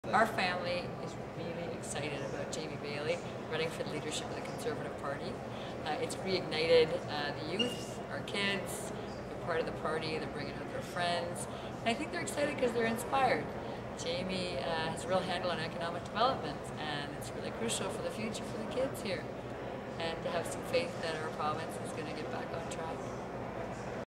Barb Stegemann Endorses Jamie Baillie for Nova Scotia PC Leader
Barb Stegemann, author of The 7 Virtues of a Philosopher Queen, with her son Victor, discusses why she's supporting Jamie Baillie in the Progressive Conservative Party of Nova Scotia's upcoming leadership race.